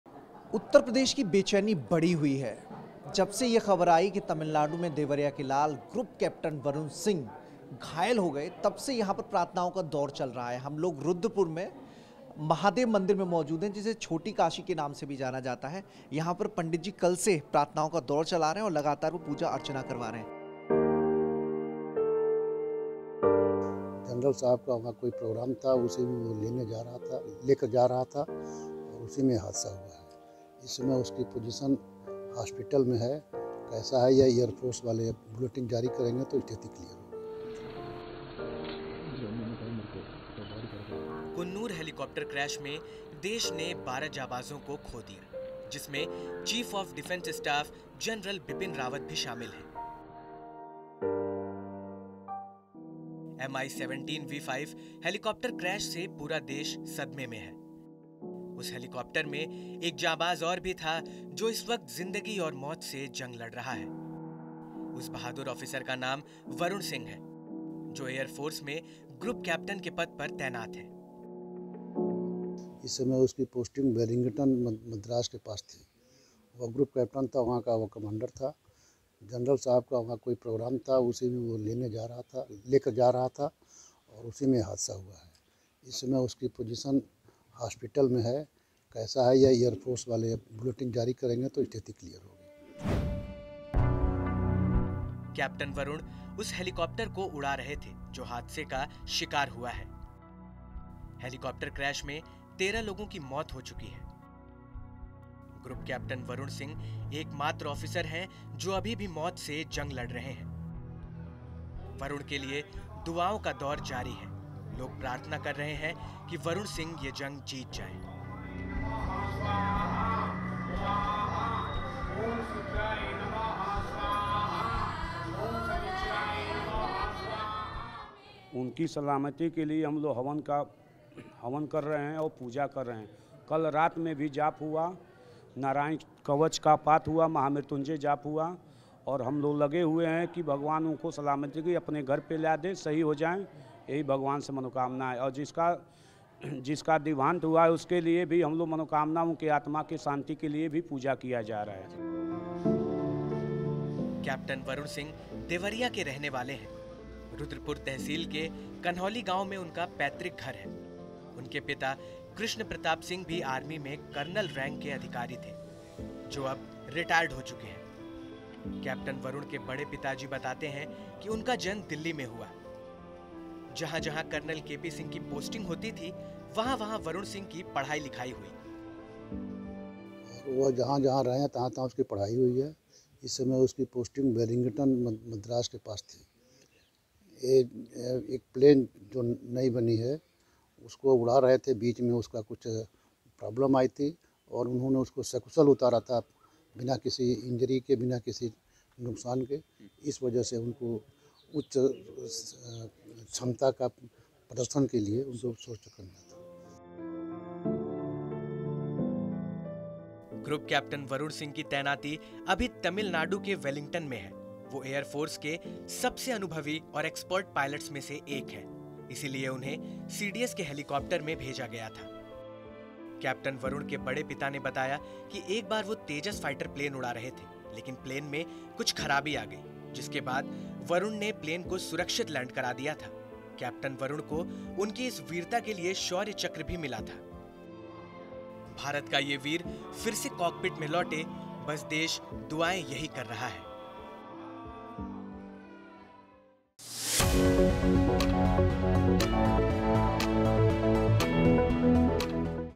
Helicopter Crash: Captain Varun Singh के लिए दुआ कीजिए । Watch Full Story
कुन्नूर हेलिकॉप्टर क्रैश में देश ने 12 जांबाजों को खो दिया... जिसमें चीफ ऑफ डिफेंस स्टाफ जेनरल बिपिन रावत भी शामिल हैं..। Mi17V5 हेलिकॉप्टर क्रैश से पूरा देश सदमे में है.. उस हेलिकॉप्टर में एक जांबाज और भी था जो इस वक्त जिंदगी और मौत से जंग लड़ रहा है..। उस जांबाज का नाम वरुण सिंह हैं... जो एयरफोर्स में ग्रुप कैप्टन के पद पर तैनात हैं।
कैप्टन वरुण उस हेलिकॉप्टर को उड़ा रहे थे जो हादसे का शिकार हुआ..।  हेलिक़ॉप्टर क्रैश में 13 लोगों की मौत हो चुकी है, ग्रुप कैप्टन वरुण सिंहGroup Captain Varun Singh एक मात्र ऑफिसर (Officer) हैं जो अभी भी मौत से जंग लड़ रहे हैं। ग्रुप कैप्टन के लिए दुआओं का दौर जारी है.. लोग प्रार्थना कर रहे हैं कि, वरुण सिंह ये जंग जीत जाएं..
कैप्टन वरुण सिंह, देवरिया (Deoria )के रहने वाले हैं.. रुद्रपुर तहसील के कन्हौली गांव (Kanhauli Village)में उनका पैतृक घर है. उनके पिता कृष्ण प्रताप सिंह भी आर्मी में कर्नल रैंक के अधिकारी थे, जो अब रिटायर्ड हो चुके हैं..। कैप्टन वरुण के बड़े पिता जी बताते हैं कि उनका जन्म दिल्ली में हुआ, जहां जहां कर्नल केपी सिंह की पोस्टिंग होती थी, वहां वहां वरुण सिंह की पढ़ाई लिखाई हुई..।
ग्रुप कैप्टन वरुण सिंह की तैनाती अभी तमिलनाडु के वेलिंगटन में है. वो एय़रफोर्स के सबसे अनुभवी और एक्सपर्ट पायलट्स में से एक हैं, इसीलिए उन्हें सीडीएस के हेलिकॉप्टर में भेजा गया था । कैप्टन वरुण के बड़े पिता ने बताया कि एक बार वो प्लेन उड़ा रहे थे, लेकिन प्लेन में कुछ खराबी आ गई जिसके बाद वरुण ने प्लेन को सुरक्षित लैंड करा दिया औऱ सभी यात्रियों को भी बचा लिया. । कैप्टन वरुण को उनकी इस वीरता के लिए शौर्य चक्र भी मिला है । भारत का ये वीर फिर से कॉकपिट में लौटे देश की दुआएं कर रहा है ।